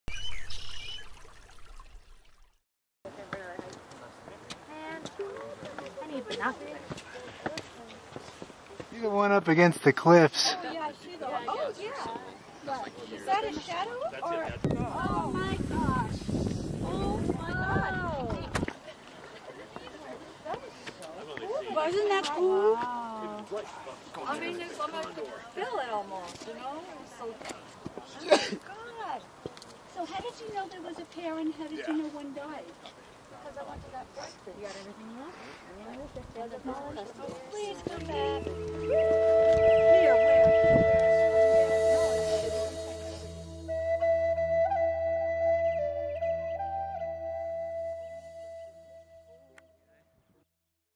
California Condor in Zion National Park HD
We have been fortunate to have had many unforgettable experiences. Here is one that will forever rate at, or near the top of our list of favorites. Listen to the people who were there. Their responses will tell you far more about the experience then the few words I type here. This footage was recorded atop Angels Landing in Zion National Park 2013.